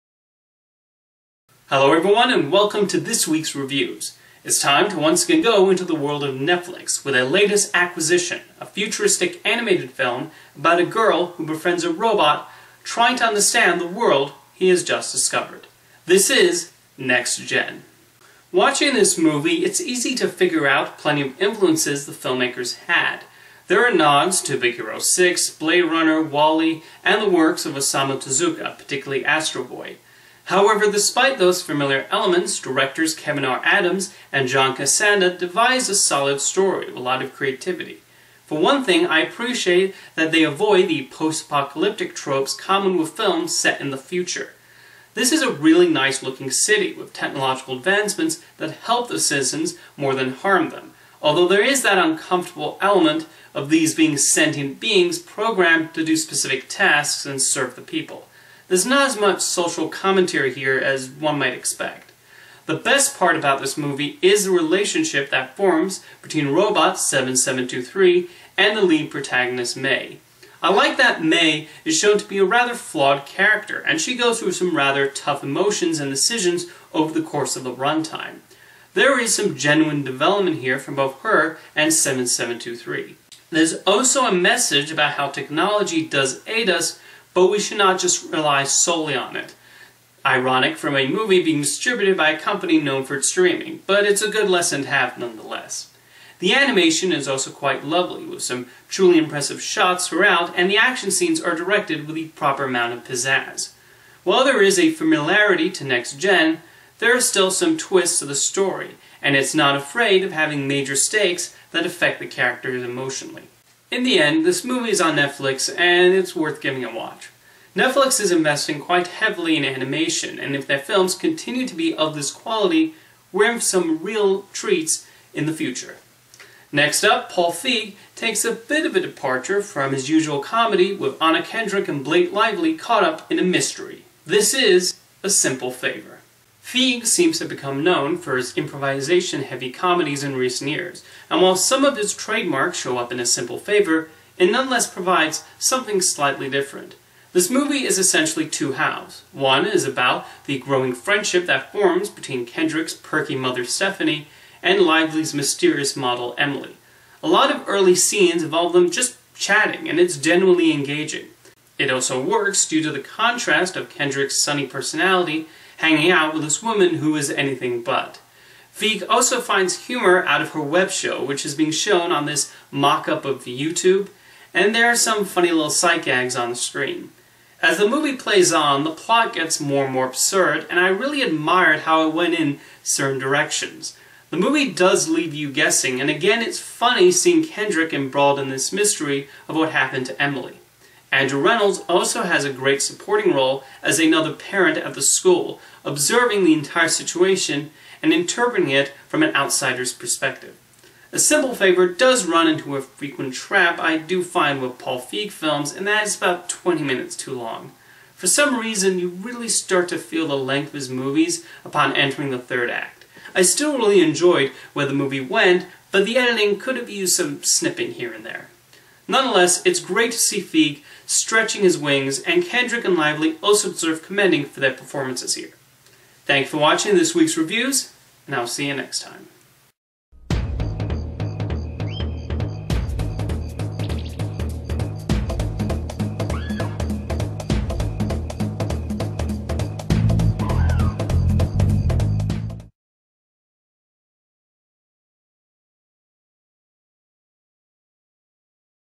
Mr. Coat's Movie Reviews: "Next Gen" and "A Simple Favor"
This week, I review Netflix's futuristic animated movie "Next Gen" and Paul Feig's dark comedy/mystery "A Simple Favor."